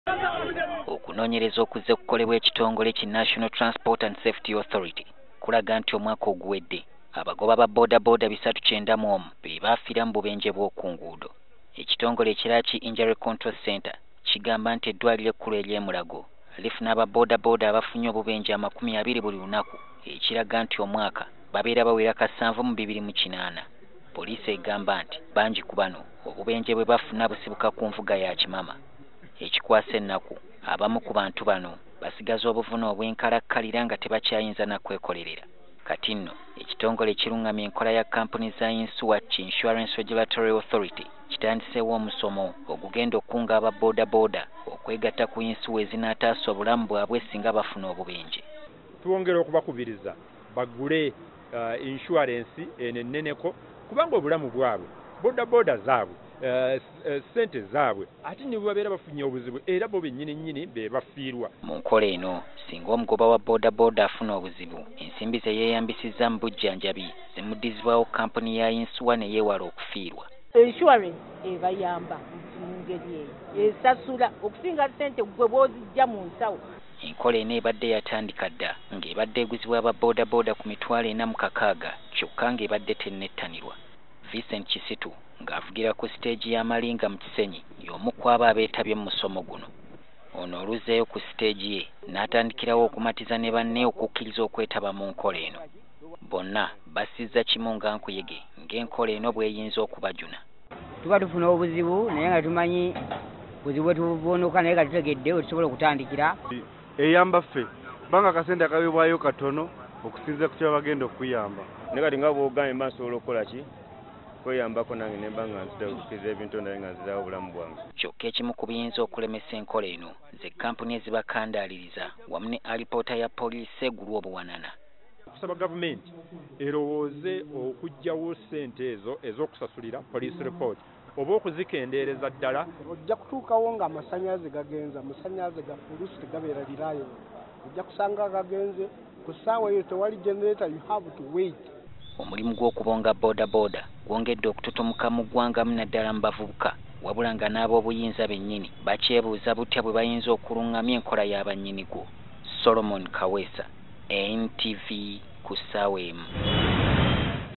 Omugaso gwa yinsuwa: Abagoba ba boda tebannagumanya
Bangi ku bagoba ba bodaboda mu kibuga Kampala ekigambo yinsuwa y’obulamu bawulira kiwulire era bangi olumala okusasula emanyiddwa nga Third Party ey’obuwaze nga bawummula.

Kino nno kiviriiddeko abafuna obuvune okuviiramu awo nga ayokya omusota so singa baba ne Yinsuwa y’obulamu bandibadde batasiibwa.

Ekitongole ekirungaamya enkola ya kampuni za Yinsuwa ki Insurance regulatory Authority kitandiseewo omusomo ogw’okubangula abagoba ba bodaboda basobole okwettanira Yinsuwa kika kino.